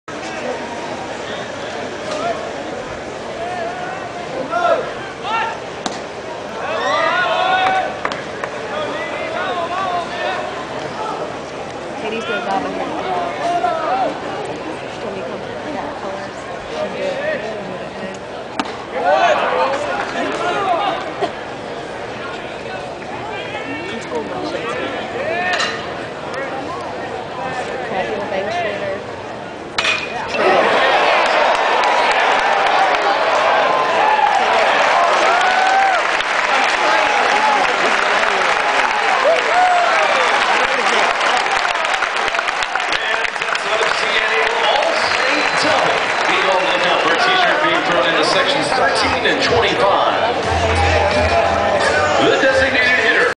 Adrian Morales hits a double vs SIU
Gamecock baseball's third baseman Adrian Morales hits a double against SIU. :)